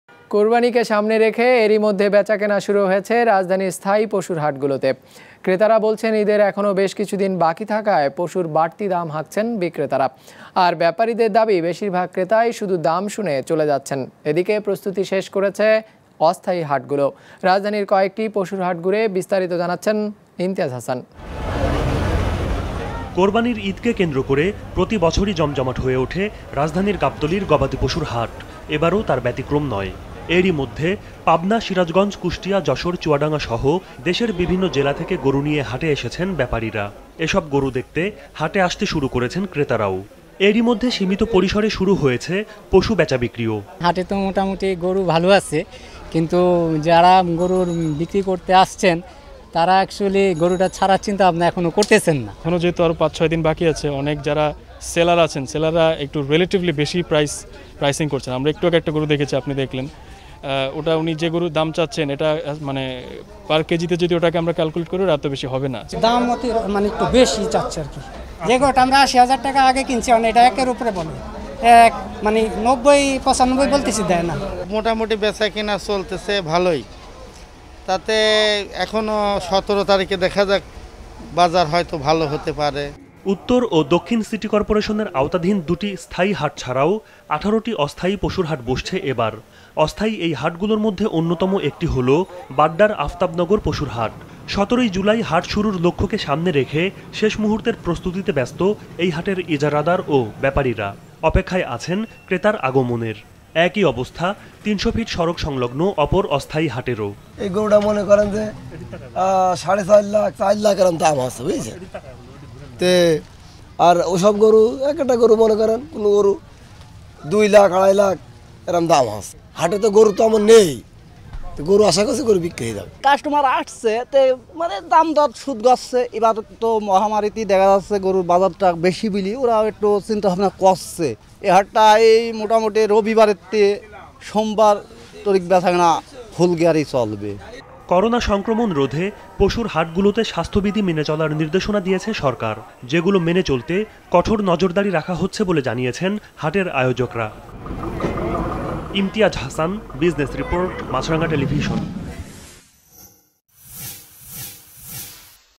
পশুর হাটে বেচাকেনা শুরু...| Bangla Business News | Business Report 2021
কোরবানিকে সামনে রেখে এরই মধ্যে বেচাকেনা শুরু হয়েছে রাজধানীর স্থায়ী পশুর হাটগুলোতে। ক্রেতারা বলছেন, ঈদের এখনও বেশকিছু দিন বাকি থাকায় পশুর বাড়তি দাম হাঁকছেন বিক্রেতারা। আর ব্যাপারিদের দাবী, বেশিরভাগ ক্রেতাই শুধু দাম শুনে চলে যাচ্ছেন। এদিকে প্রস্তুতি শেষ করেছে অস্থায়ী হাটগুলোও...

| Bangla Business News | Business Report 2021

Camera: Bacchu Mridha, Mizanur Rahman.
Editing: Mahbub Alam Shohag.
Graphics: Debashis Roy.
Post Production: Mahafujul Hasan Sajib.
Producer: Emdadul Jahid.
Channel: Maasranga Television.